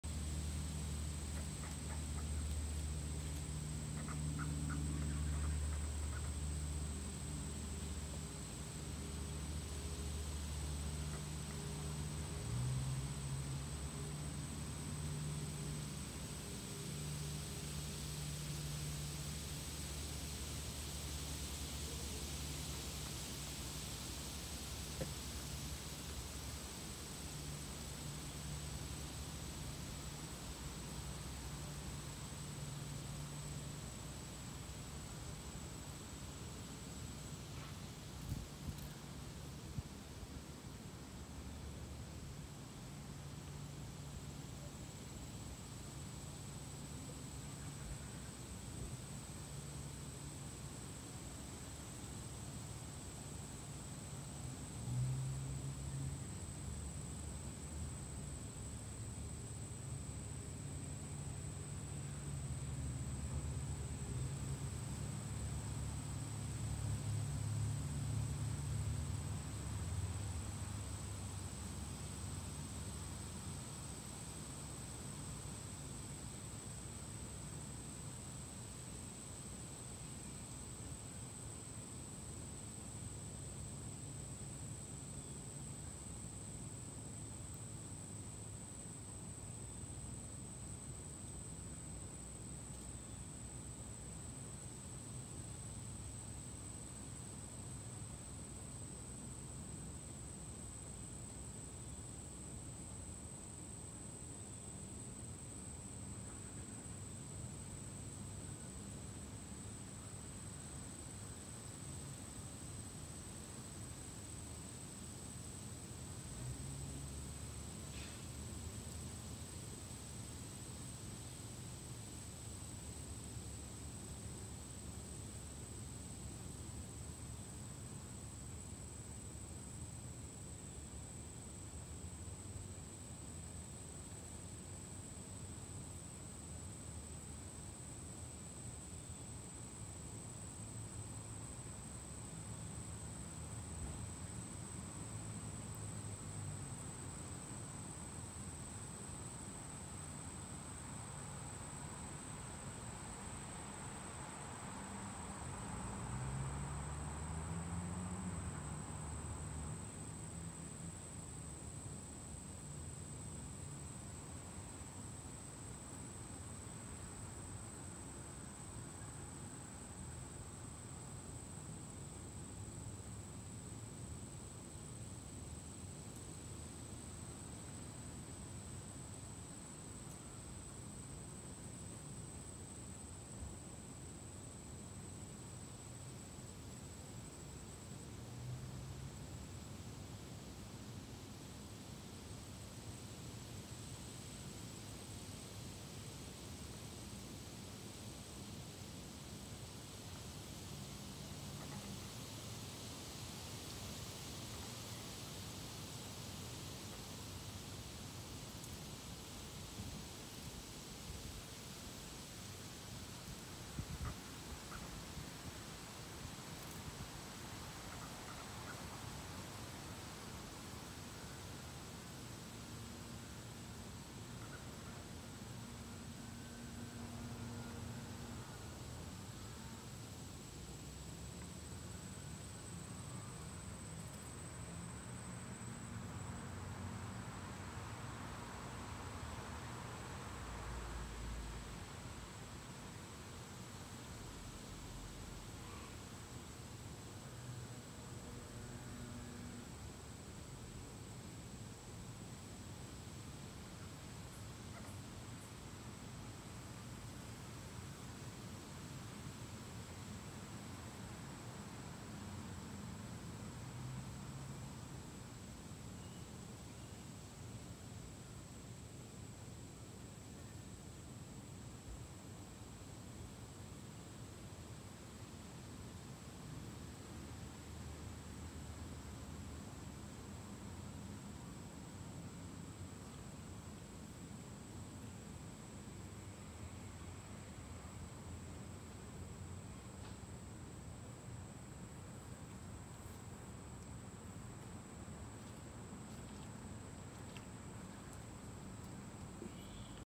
Five Minutes of Five Flower Pots
The dipladenias on my back patio gently swaying in the lunchtime breeze.